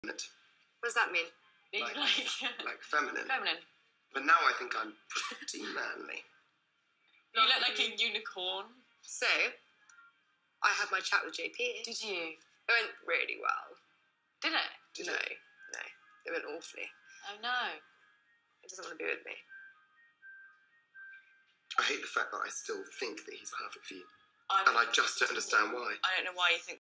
Binky Felstead - Bar Necklace - Sparkling Jewellery
Binky Felstead Wearing Sparkling Jewellery - As seen on TV. Megan is wearing our Solid Bar Collection. Our solid bar necklace in gold.